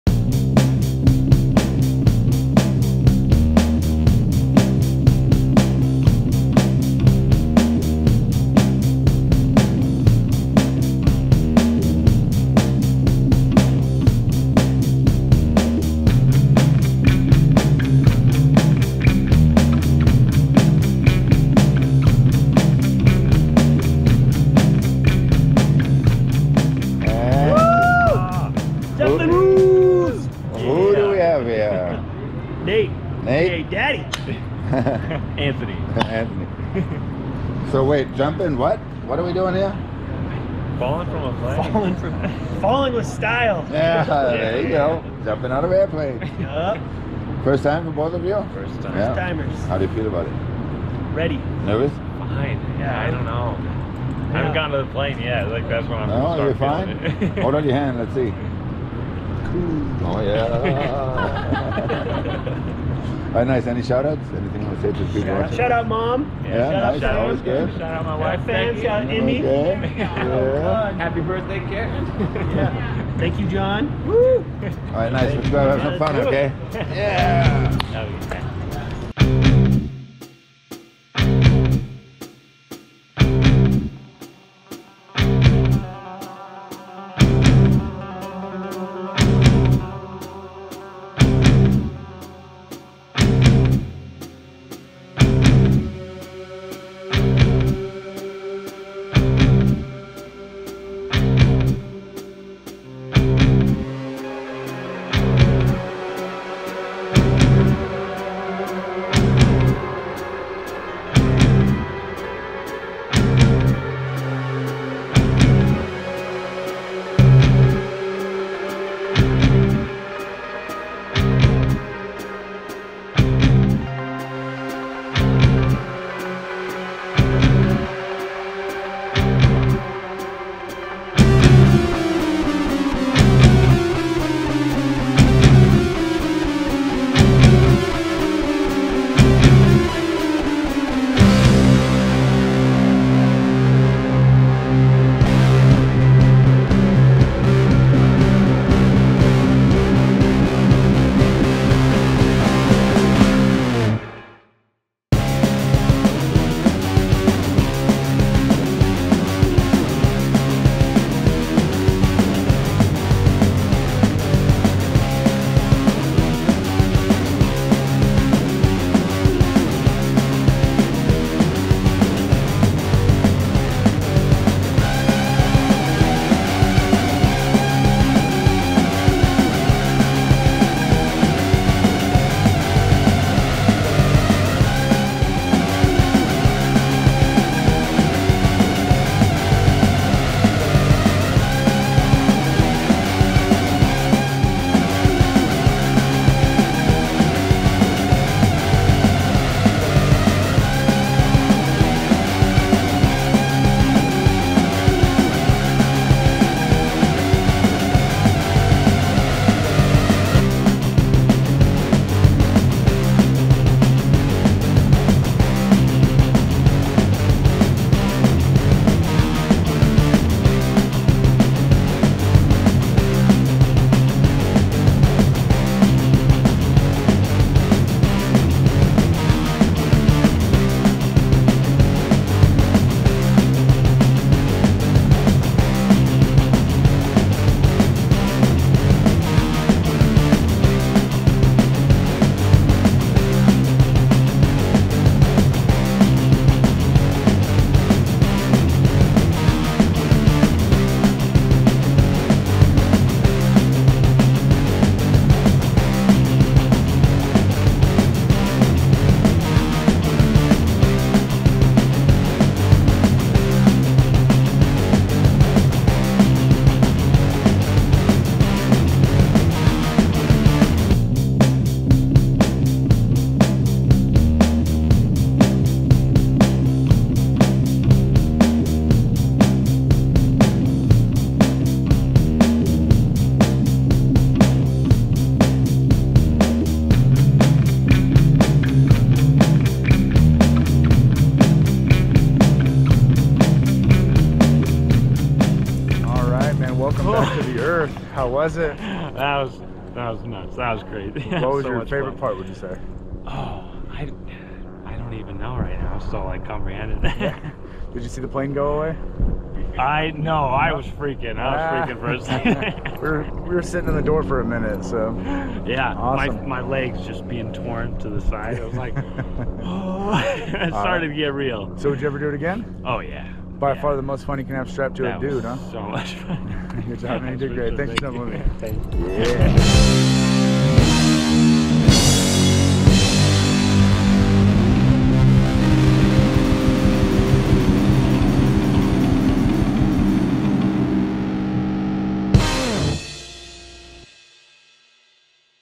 SKYDIVE - Lake Tahoe
Woke up on Friday, July 22, 2022 in South Lake Tahoe, ate breakfast, had a mimosa, and was invited to skydive in two hours... So I did! Thank you Batman! What an experience!